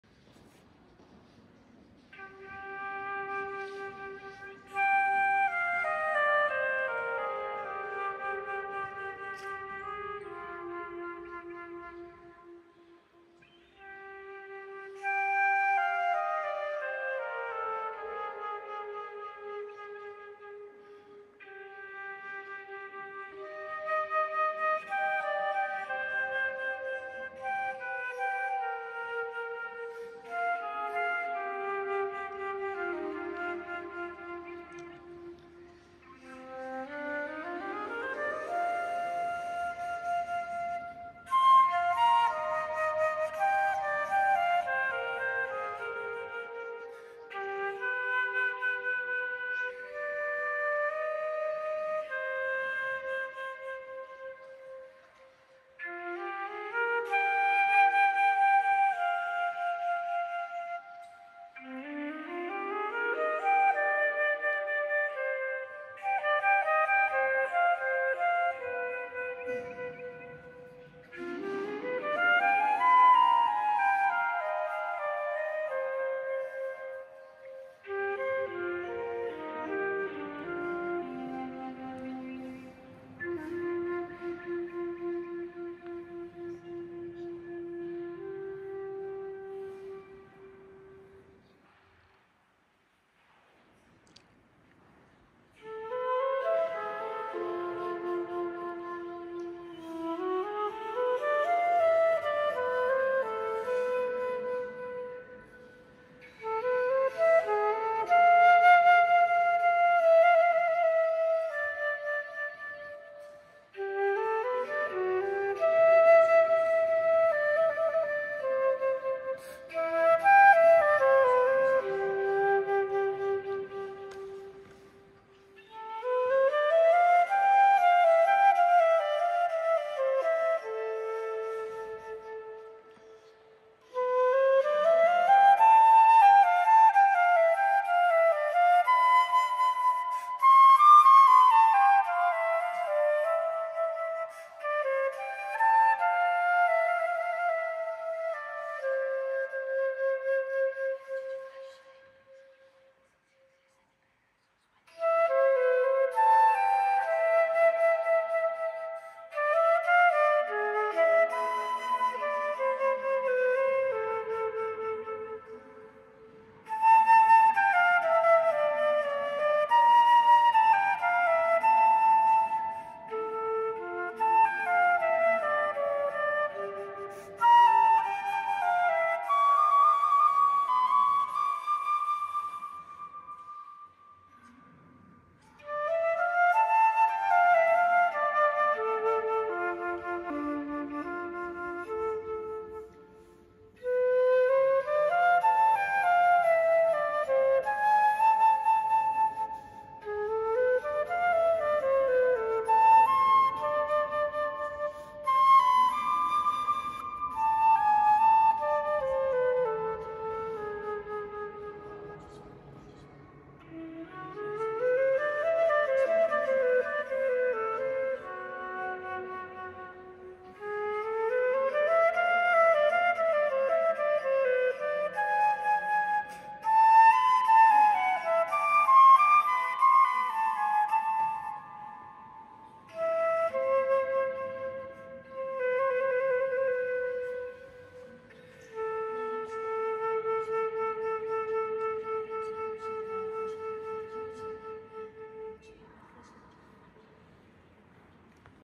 Improviso
Na esplendorosa igreja da cidadezinha de San Benedetto del Po, Itália, estou fazendo um improviso livre onde surgiu uma citação de melodia do mestre, sempre genial, Tom Jobim.